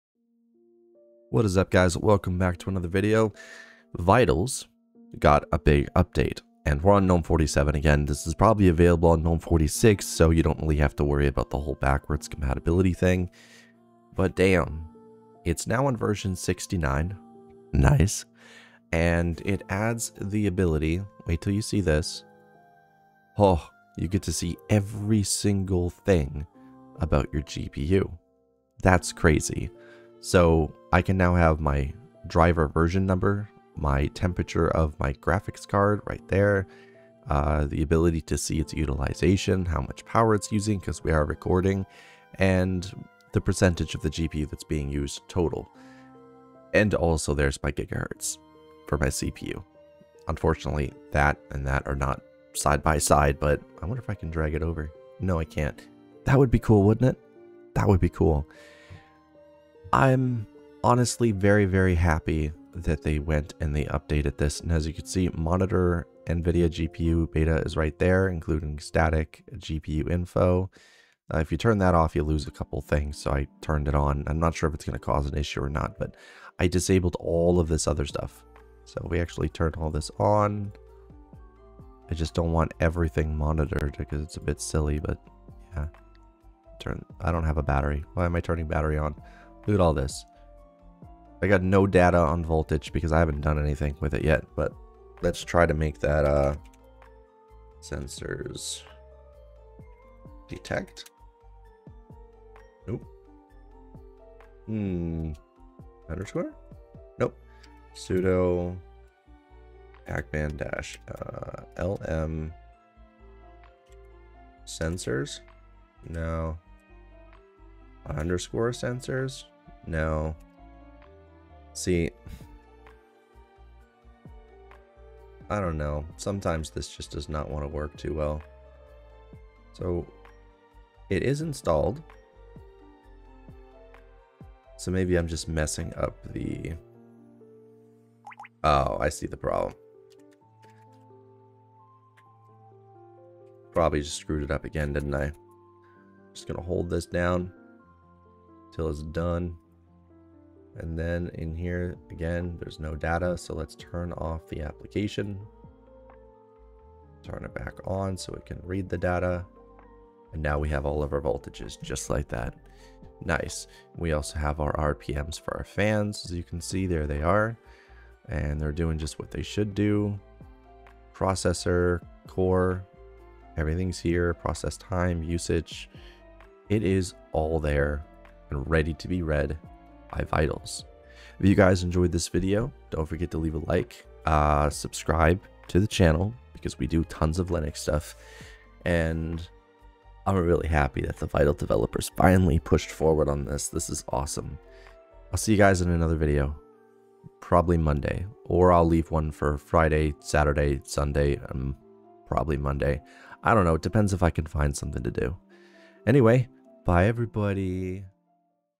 Vitals Gnome Extension Got a Huge Update
SPECS Below:
• OS: Fedora 40 Workstation
• OS: Nobara 40
• OS: CachyOS
• Gnome 46
• CPU: Ryzen 7 5900X (4.9GHz)
• Memory: 49 GiB DDR4 3600mhz
• Storage: 22TB
• GPU: RTX 4070